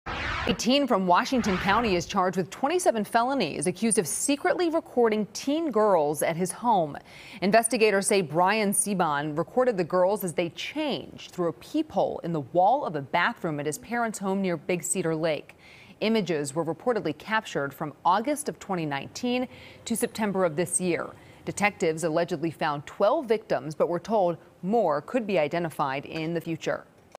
Washington County 17-year-old recorded girls changing, sheriff says | FOX6 News Milwaukee
Prosecutors say a former Kettle Moraine Lutheran student used cellphones and a bathroom peephole to take photos and videos of 12 girls, many of them his friends who came over to swim at his house.